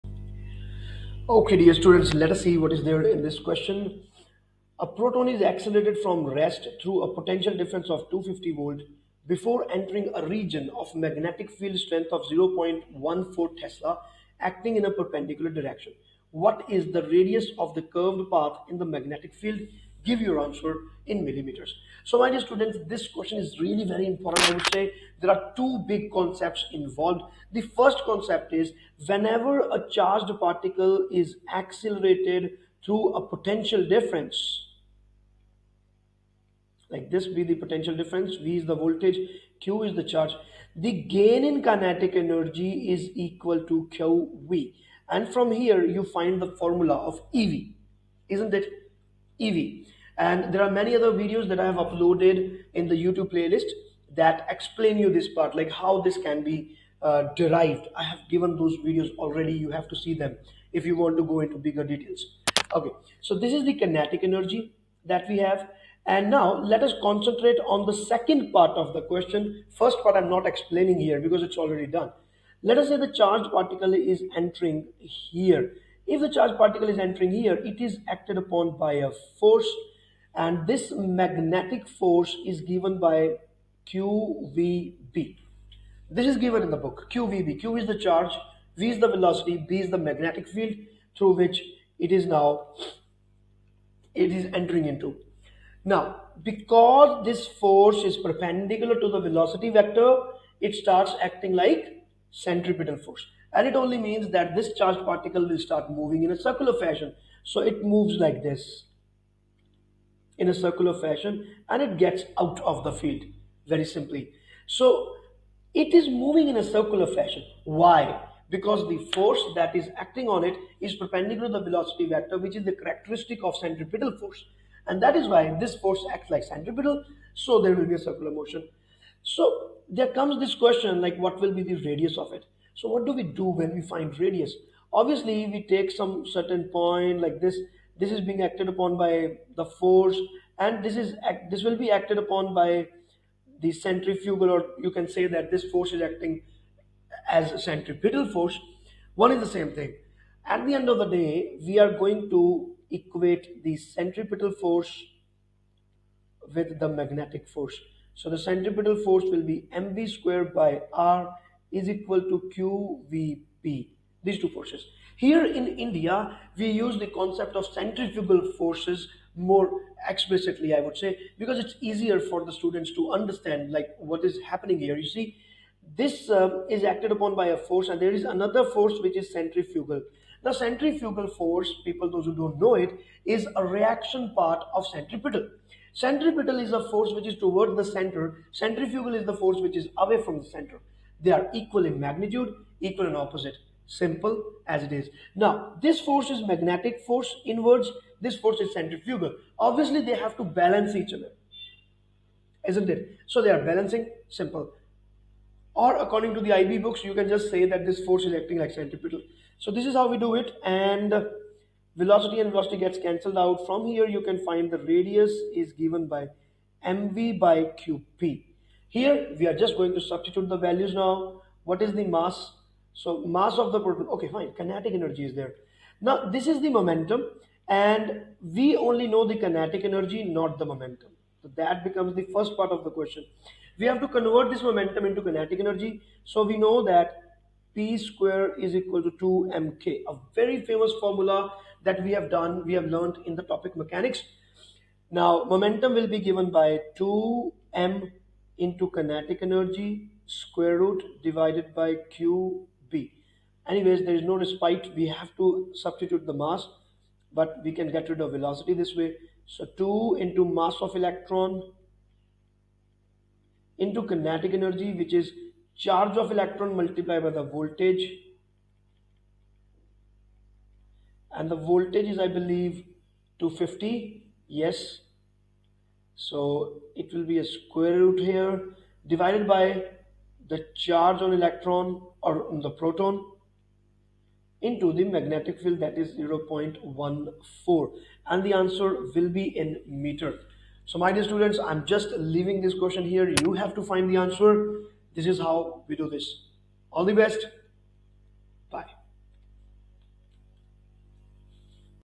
A proton is accelerated from rest through a potential difference of 250V before entering a region
A proton is accelerated from rest through a potential difference of 250V before entering a region of magnetic field of strength 0.1 T acting in a perpendicular direction. What is the radius of curved path in the magnetic field give your answer in mm.